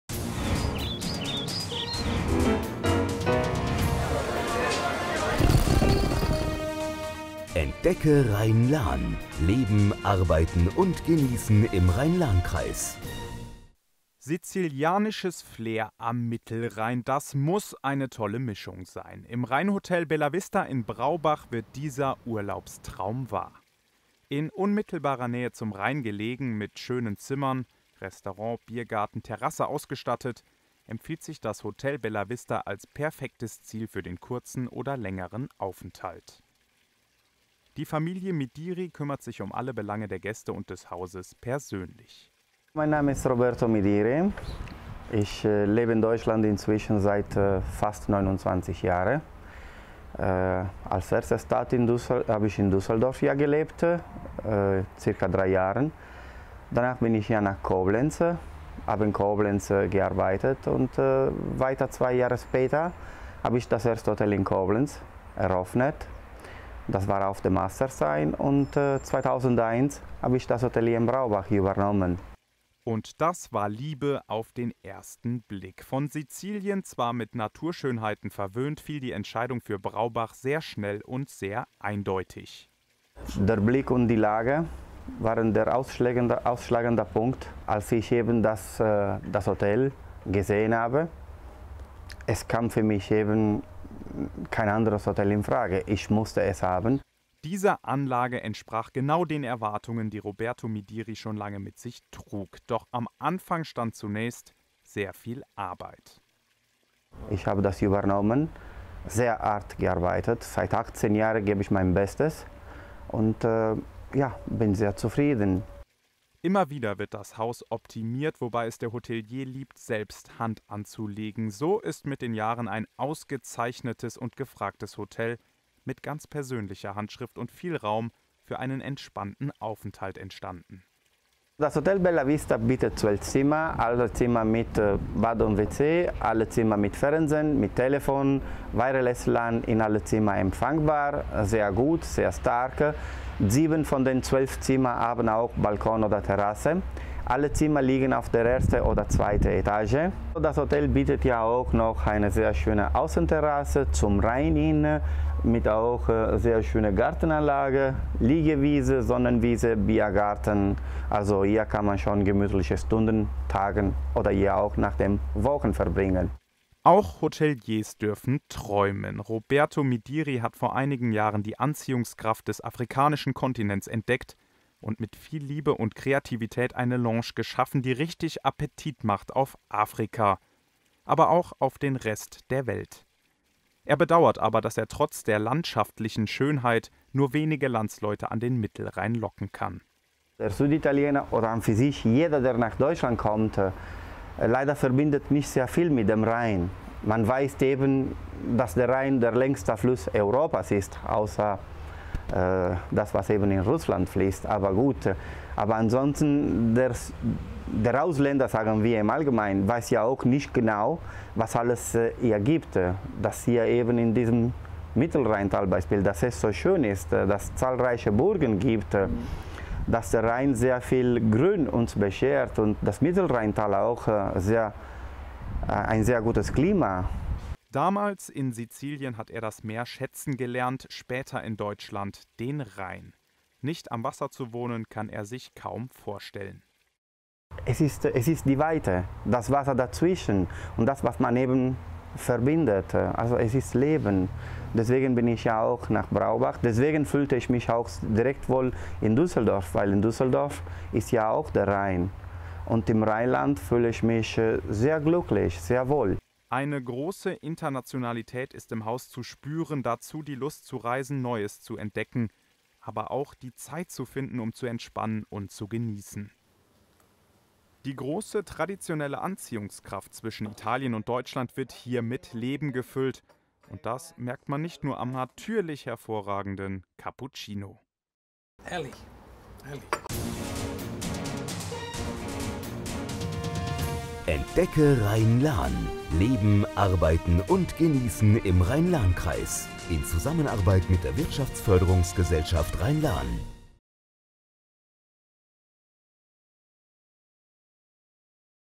739 - Hotel "Bellavista" in Braubach
Dieses Hotel macht seinem Namen wirklich alle Ehre. Von der großzügigen Terrasse des „Bellavista“ in Braubach hat man einen einmaligen Blick ins Rheintal und auf die Marksburg. Dazu moderne Zimmer, italienische Küche und die engagierte Familie Midiri, ein idealer Platz zum Bleiben oder als Ausgangspunkt für spannende Ausflüge.